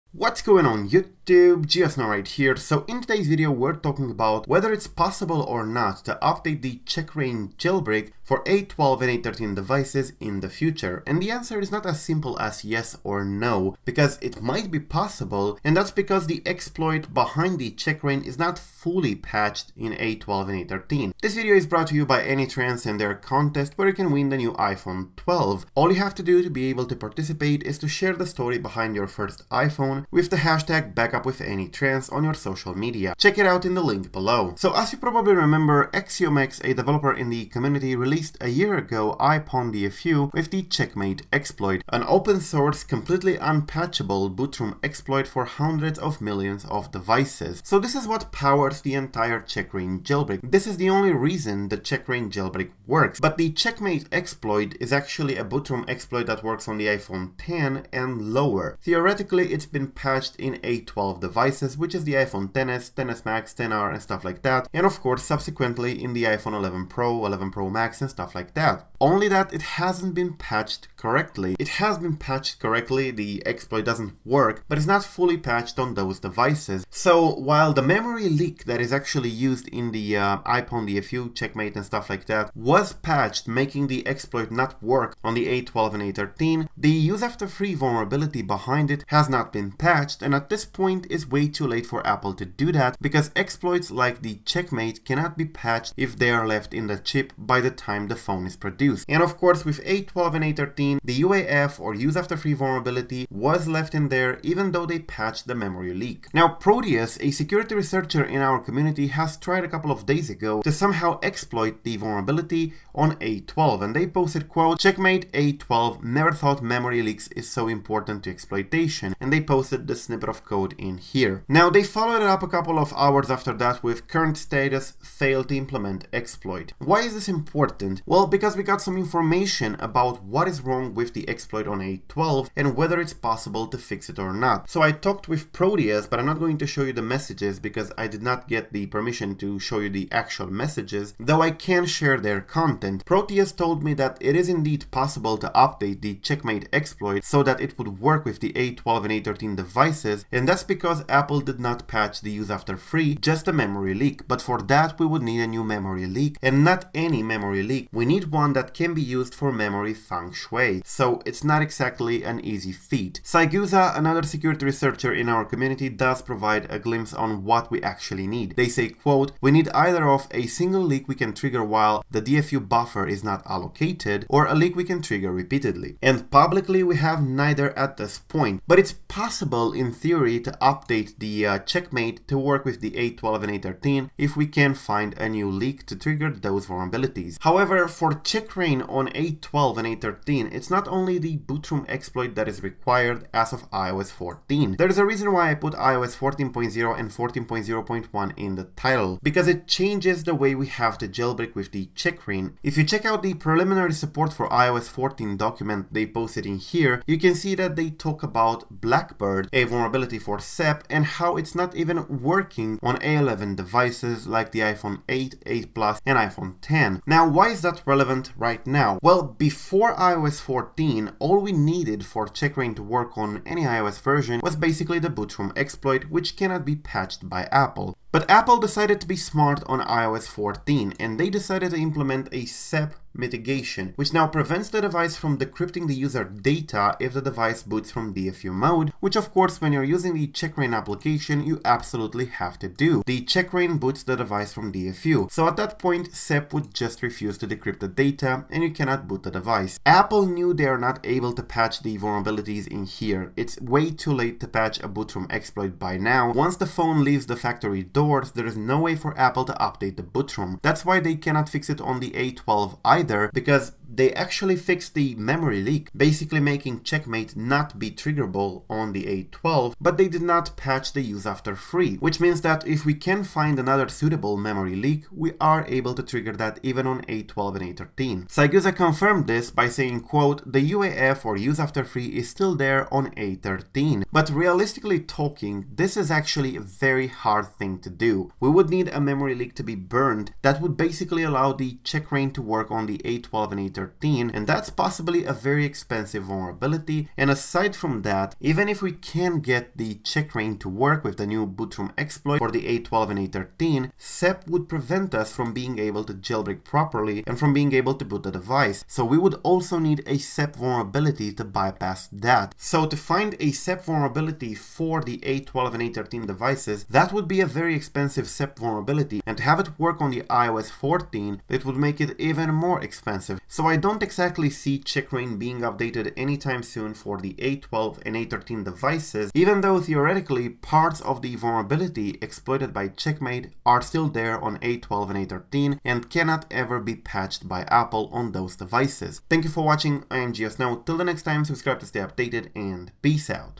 iOS 14.0.1 / 14 JAILBREAK: Can CheckRa1n Be Updated For A12 / A13? (Explained)
Today we're discussing whether the CheckRa1n Jailbreak can be updated to support A12 and A13 devices in any way. The A13 and A12 devices, are iPhone XS, iPhone XS Max, iPhone XR, and iPhone 11 / SE 2020. By default CheckRa1n only supports iPhone X and lower (A11 and lower), so would it be to somehow update CheckRa1n in the future to work with the rest of the devices as well? Maybe!

You see, CheckRa1n Jailbreak is built on top of CheckM8, a very powerful bootrom exploit that cannot be patched by Apple that was released by developer @axi0mX. While the memory leak has been patched by Apple on A12, the UaF was not, thus leaving a small possibility for CheckRa1n.

~ GeoSn0w

▶ CheckRa1n logo by CheckRa1n Team